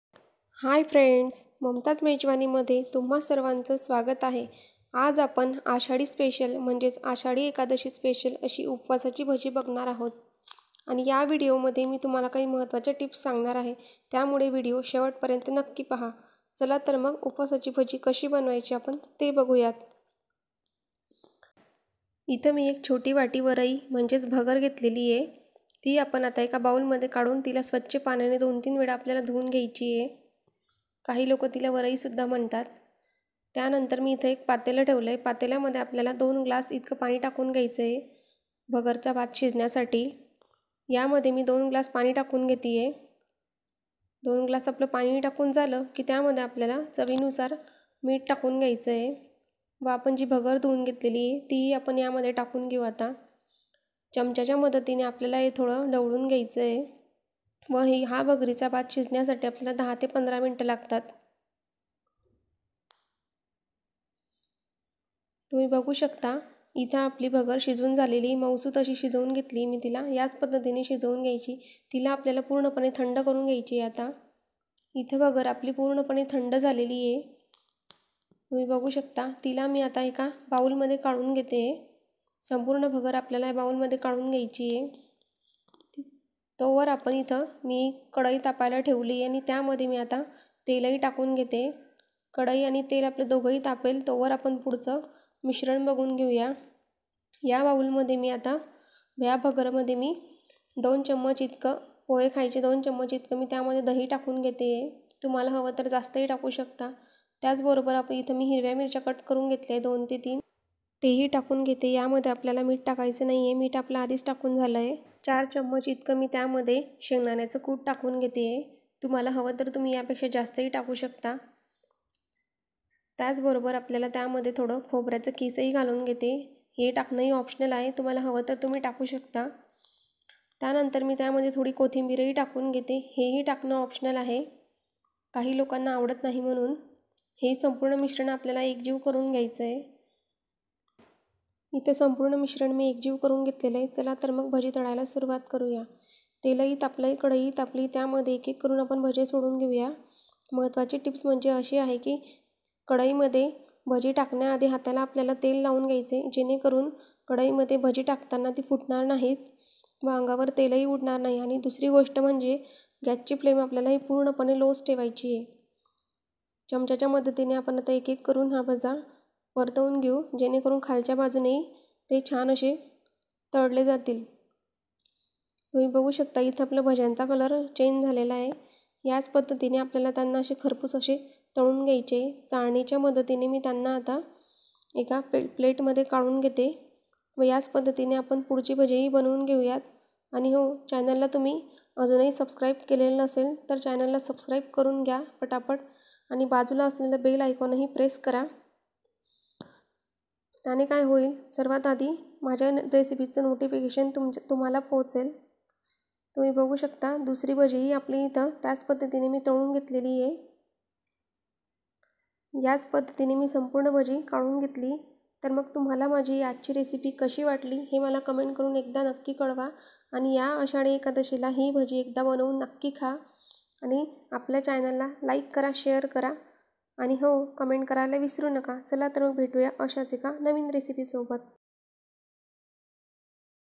आषाढी एकादशी स्पेशल कुरकुरीत उपवासाची भजी | Upvasachi bhaji | वरईची भजी | भगरची भजी
Hi friend..
पुन्हा एकदा मी आज तुमच्यासाठी एक नवीन रेसिपी घेऊन आलीय खास अशी कुरकुरीत आणि खुसखुशीत उपवासाची भजी,वरची भजी

गोवन पध्दतीची चवळीची भाजी /opN5SvAp5X8